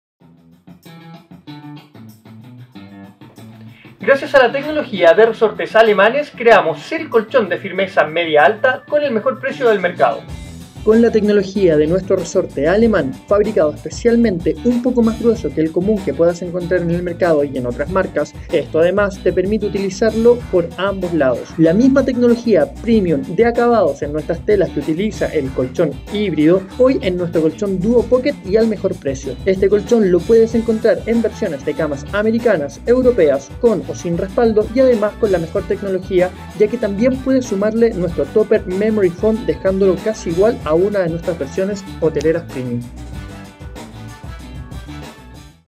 DUOPOCKET FUNCIONAL
Ven a conocer nuestro nuevo colchón DUOPOCKET  y prueba una nueva forma de descanso con drimkip.  Av. Luis Pasteur #5439, Vitacura. Sabado y Domingo abierto de 11 a 19 hrs.